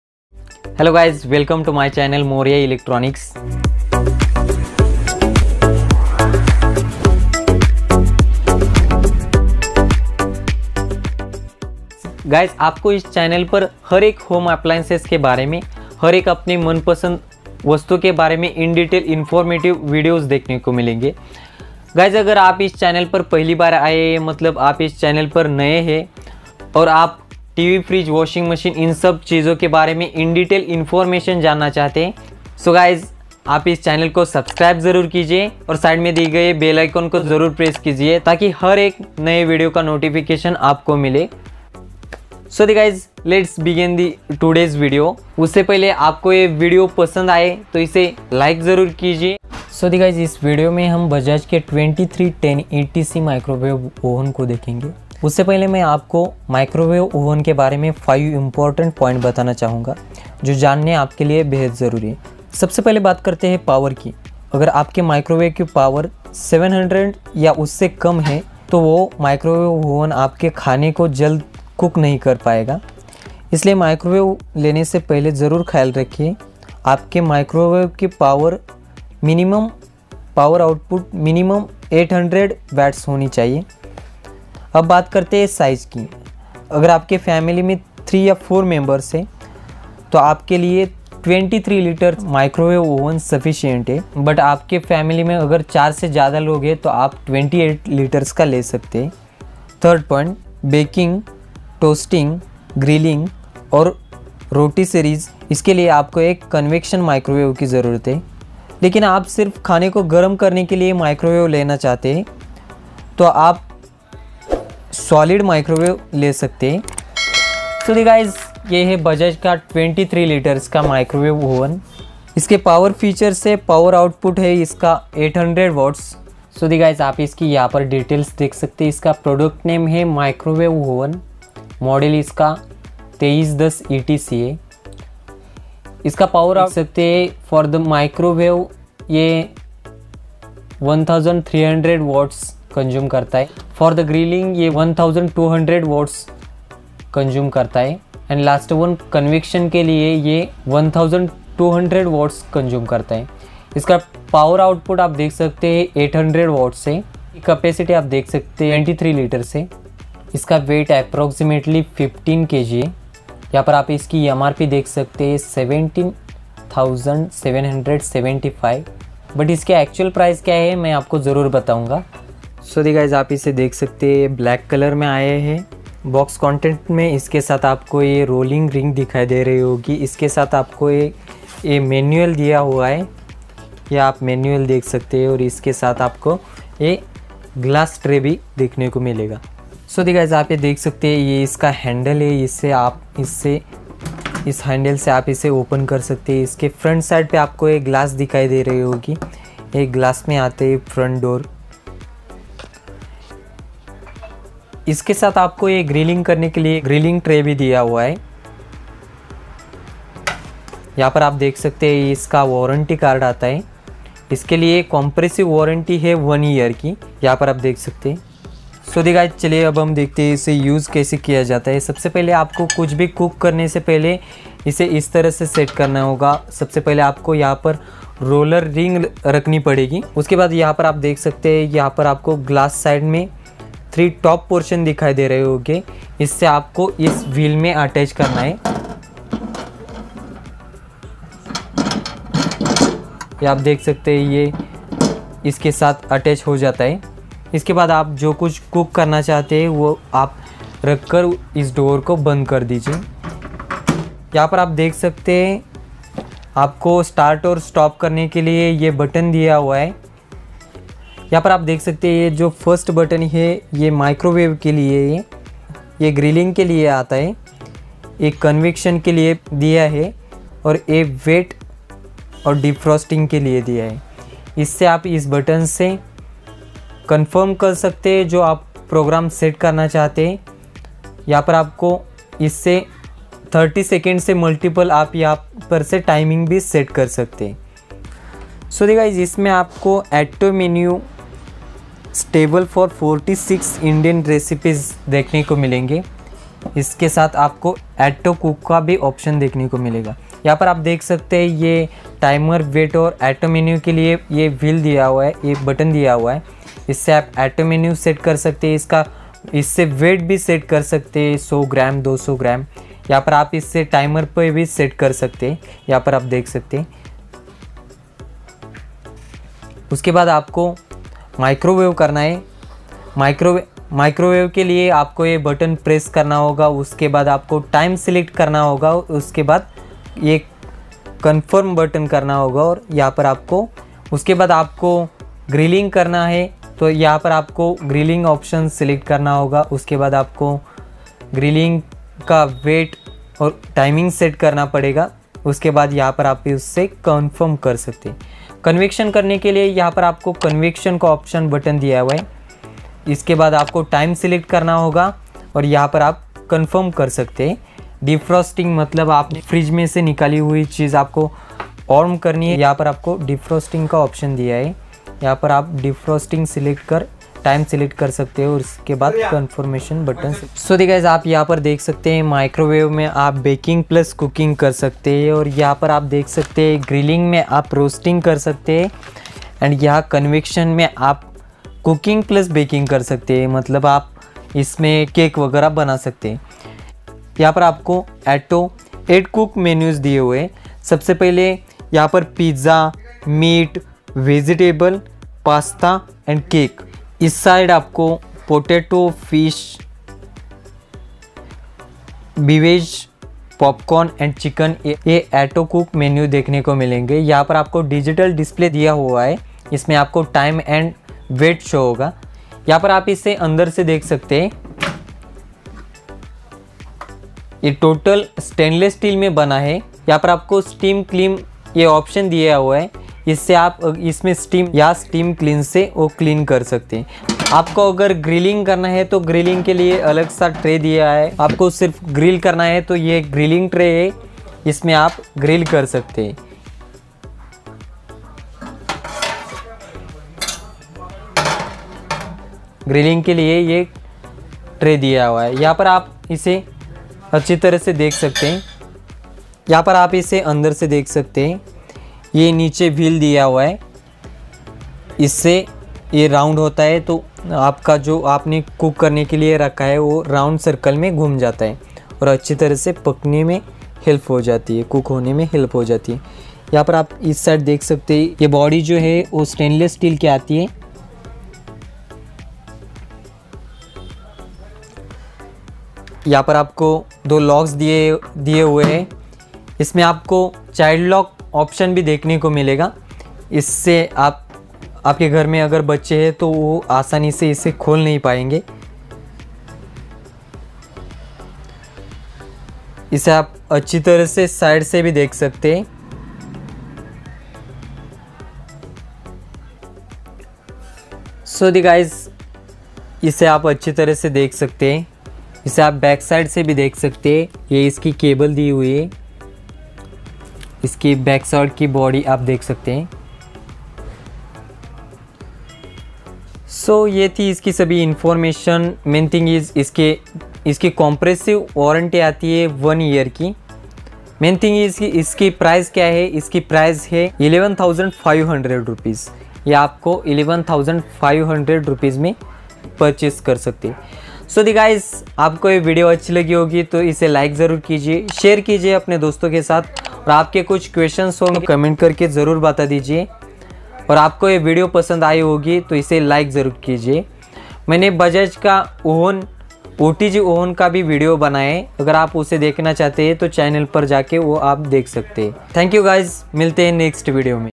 ⚡ Bajaj (2310etc) microwave oven ⚡ best microwave oven in india ⚡ Indetialed review and Demo ⚡
In this video you will get all the information about the bajaj 2310etc microwave oven.This the best microwave oven . please watch complete this video then you will get all the details.also I explained how to use this microwave.

                 Tal- khatav
                  Dist :- satara .

Milate hai next video me 🙏🇮🇳👍.